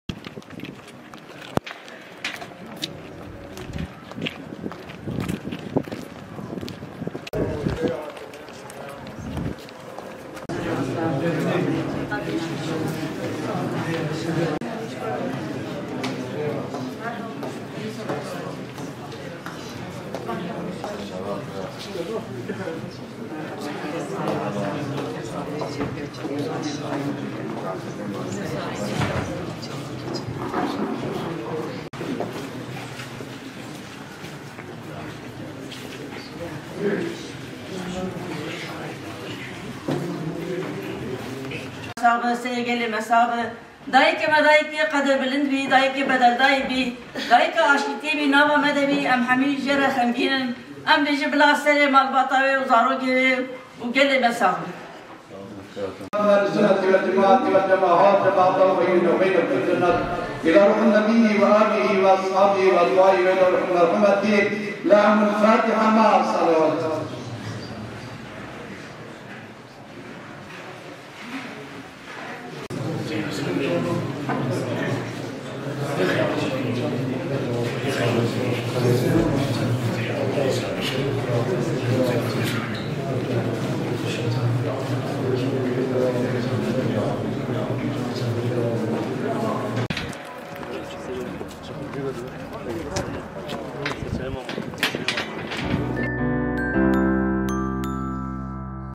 BI GIRSEYÎ SERDANA ŞÎNA DAYIKA AŞTIYÊ HAT KIRIN
Serdana şîna Dayika Aştiyê Kumrî Oner ku jiyana xwe ji dest dabû hat kirin û hat destnîşankirin ku wê xwedî li têkoşîna wê bê derketin.